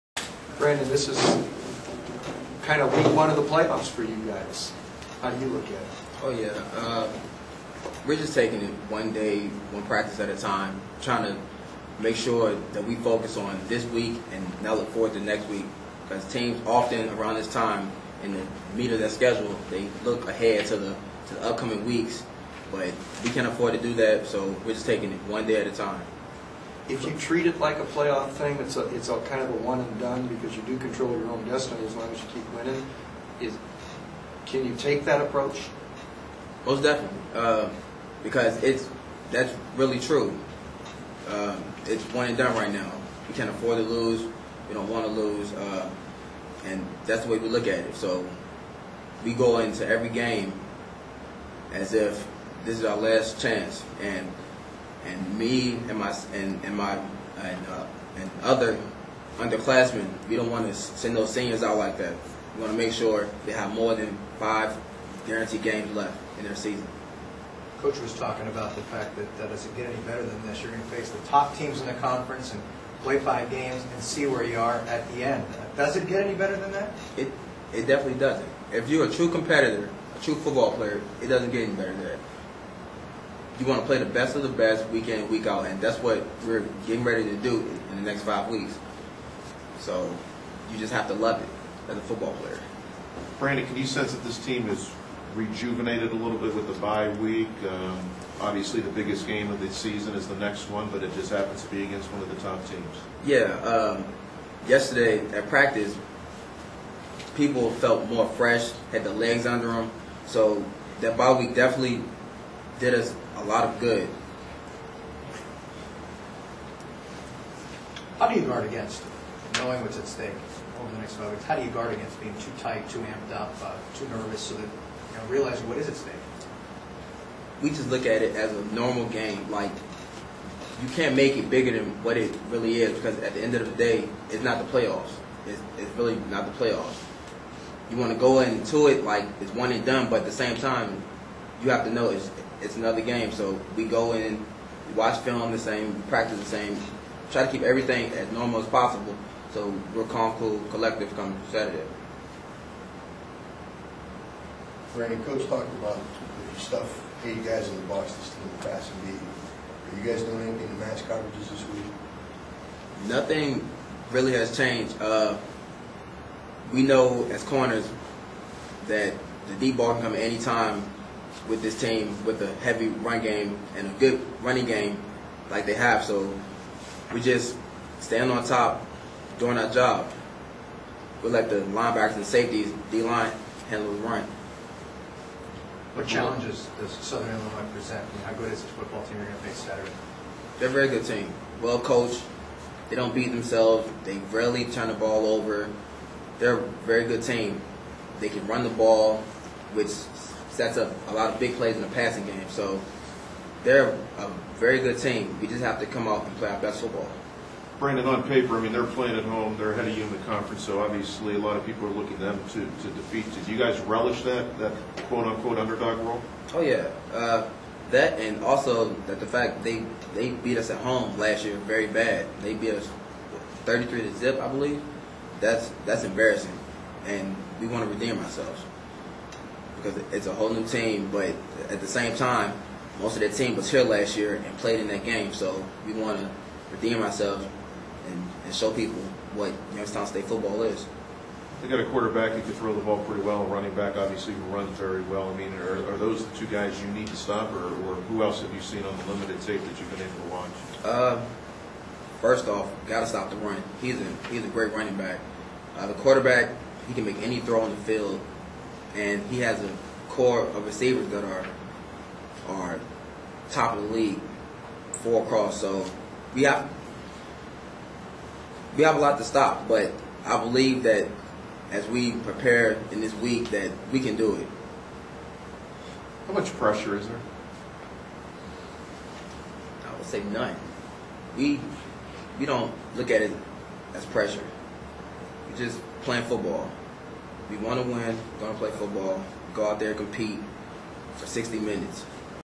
YSU Cornerback Brandian Ross | Oct. 19, 2009
Youngstown State junior cornerback discusses the Penguins' upcoming game at third-ranked Southern Illinois.